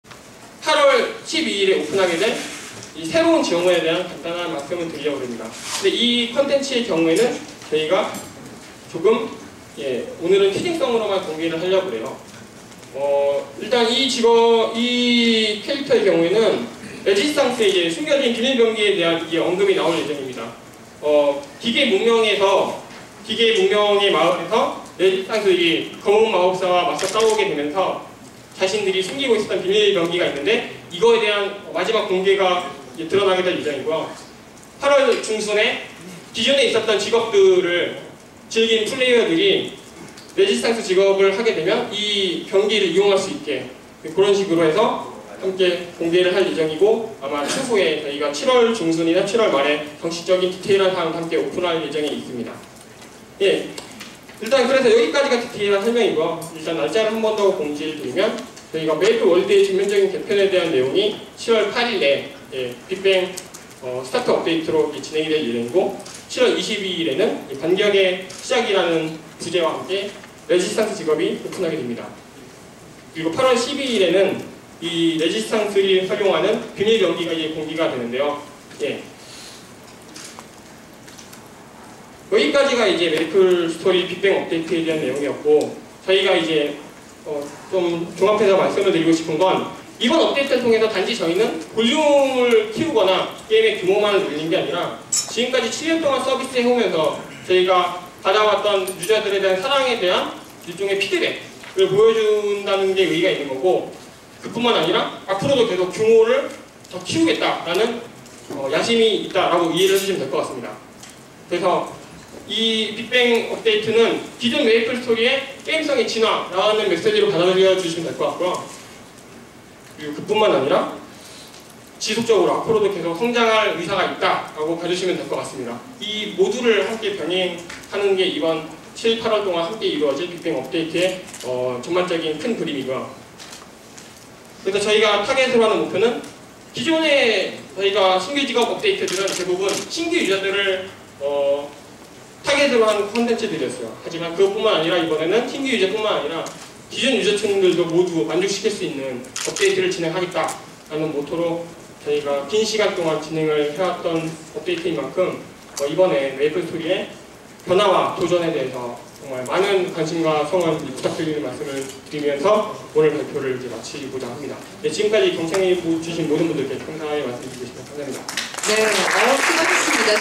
'Maple Story BIGBANG' Press Conference : Part 7/7
Nexon : Maple Story BIGBANG Press Conference - Director Hanbyul Oh's Presentation about Maple Story BIGBANG Chapter 3 New Support Forces August 12 2010 (Record : Acrofan)